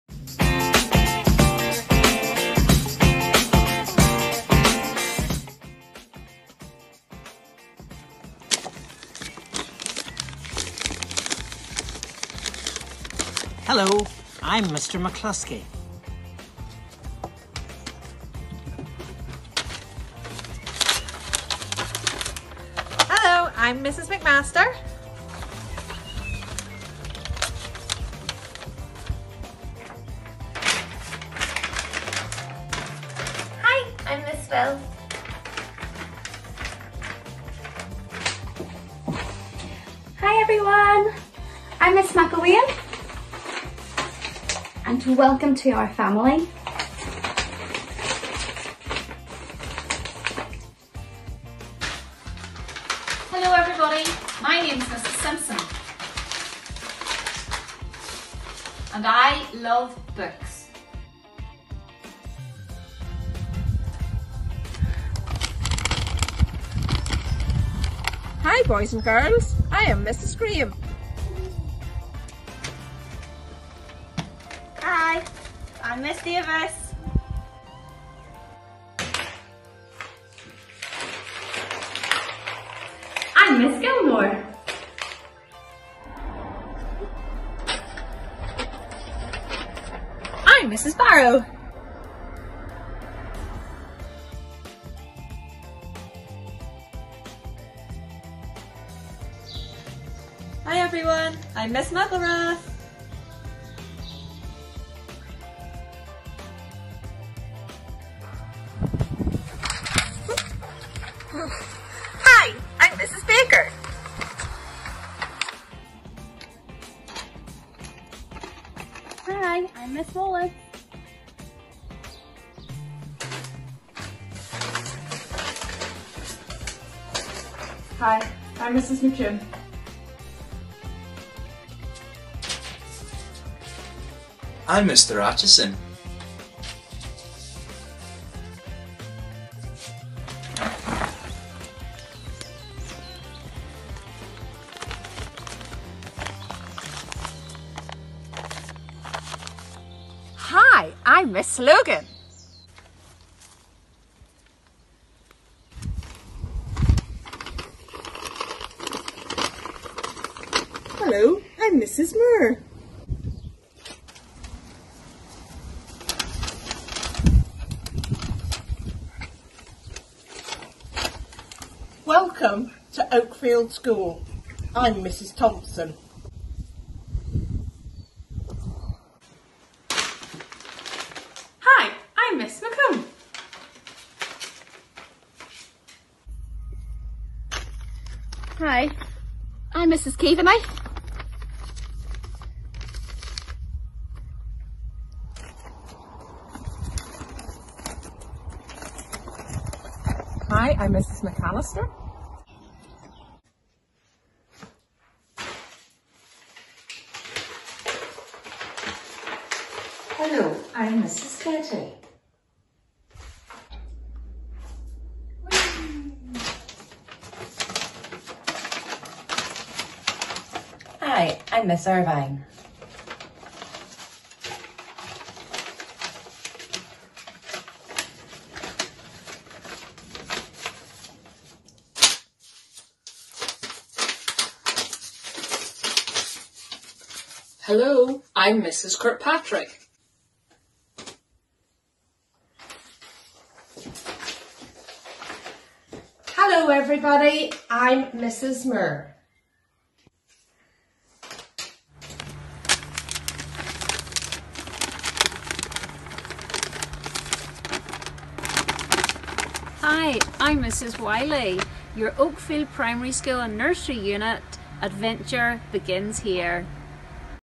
Fly High!
Journey with the Staff at Oakfield Primary School Nursery Unit.